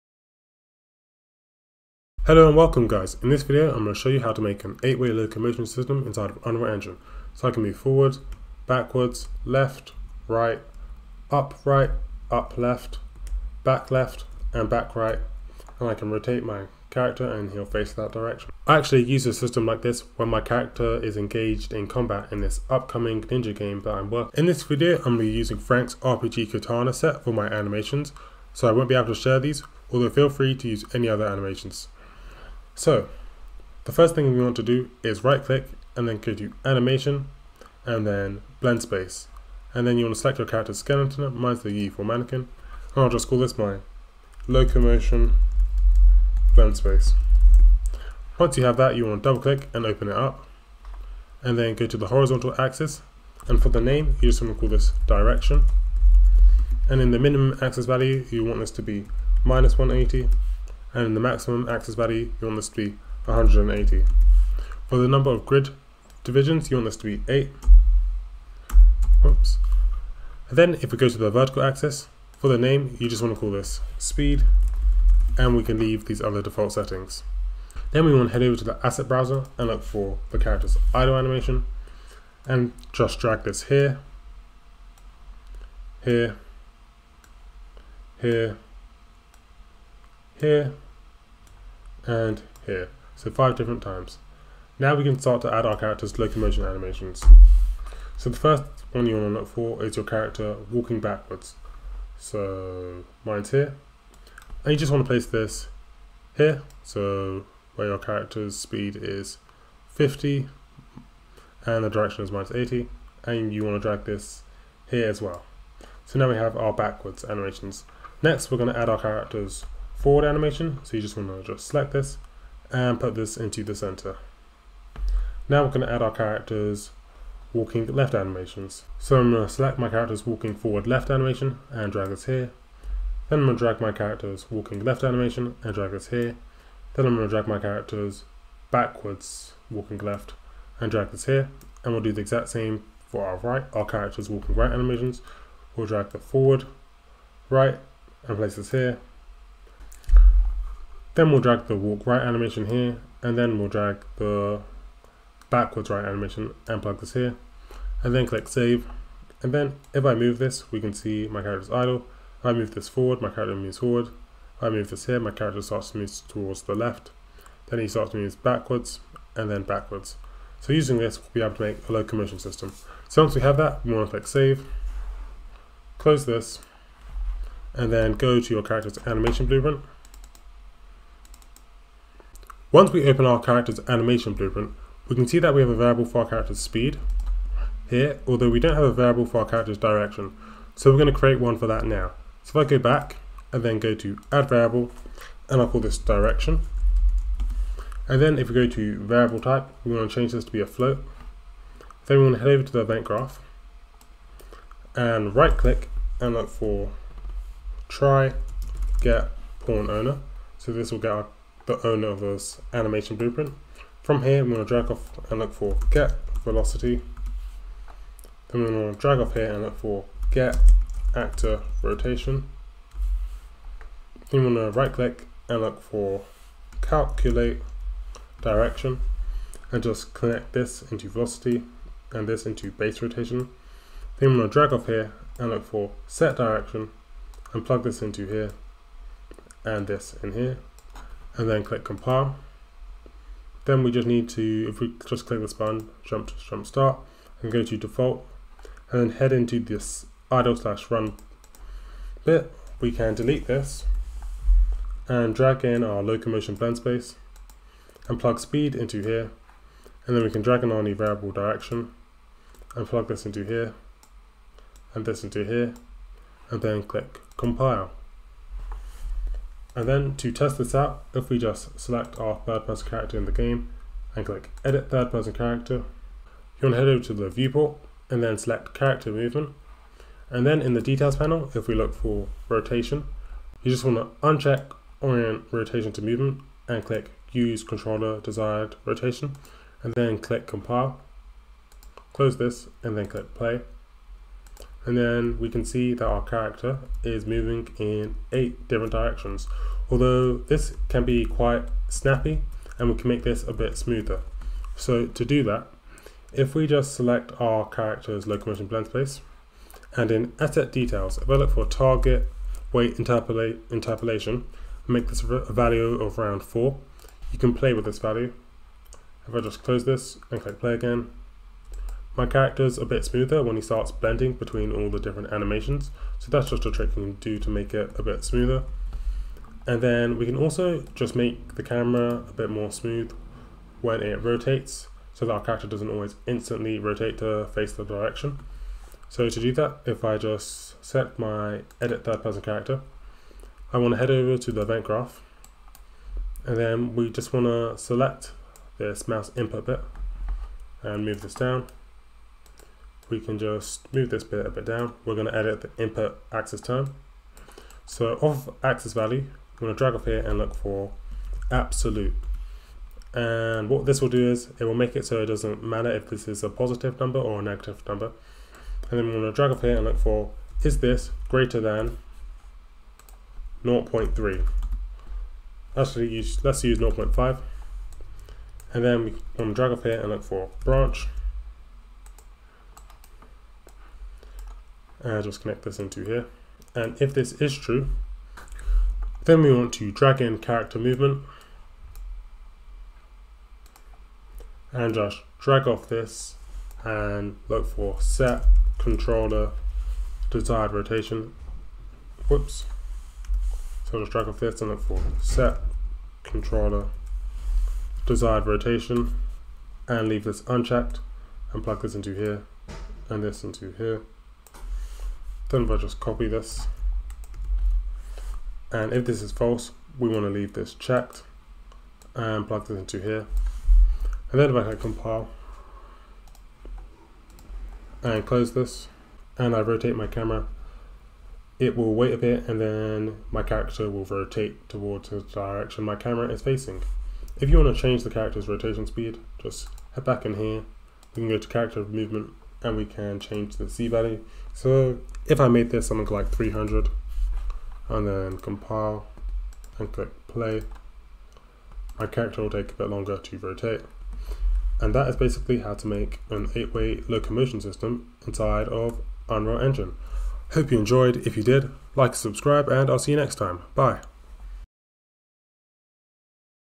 How To Make A Locomotion Movement System in Unreal Engine
In this video I go over how to make a locomotion movement system inside of unreal engine and I also give a very early sneak peak of a project I am working on